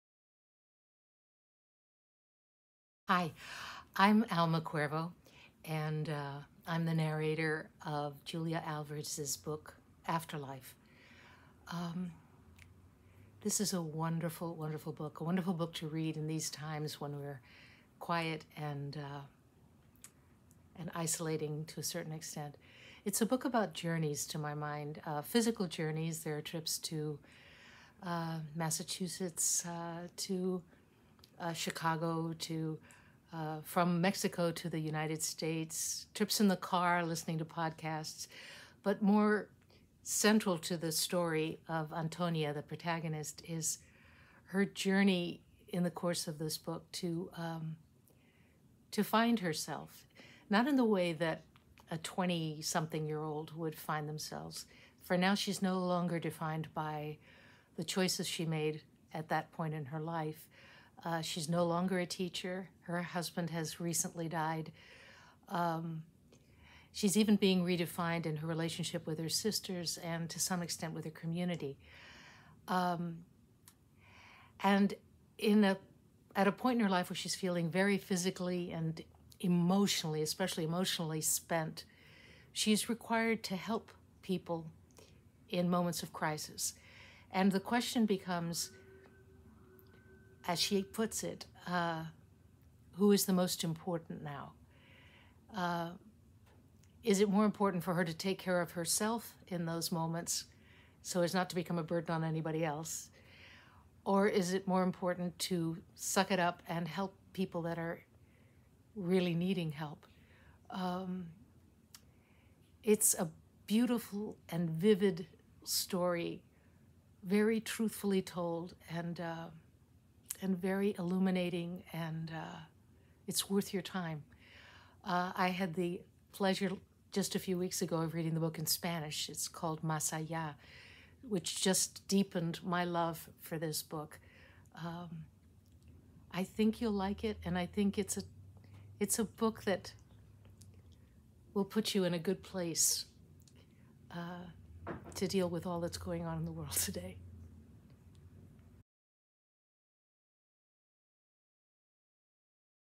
Behind the Mic: Narrator Alma Cuervo on Afterlife
Narrator Alma Cuervo tells audiobook listeners about narrating AFTERLIFE by Julia Alvarez. Alma Cuervo's warm, even-keeled performance guides listeners through the interwoven themes of love, loss, and compassion in this beautifully wrought new novel. Read the full review at AudioFile Magazine